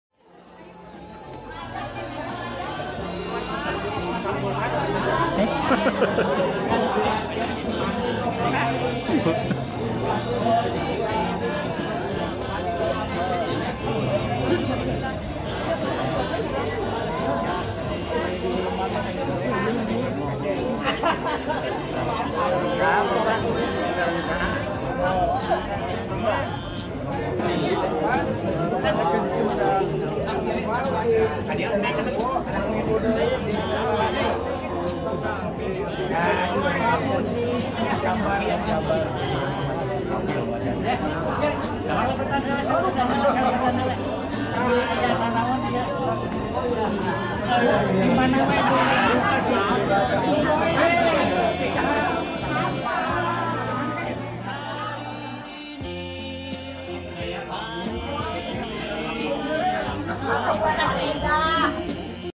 Pertemuan Guru & Anak didiknya setelah 55 tahun berlalu
Beliau adalah Pak Muhidin, seorang staff pengajar Sekolah Pertanian Menengah Atas (SPMA) Tanjungsari yang mengajar pada tahun 1960an. Kebetulan ayah saya diajar & dididik oleh beliau.
Pada acara reuni akbar SPMA yang diselenggarakan tanggal 7 Désémber 2019 beliau menyempatkan diri untuk hadir. Alhamdulillah beliau sehat di usianya yang sudah 88 tahun.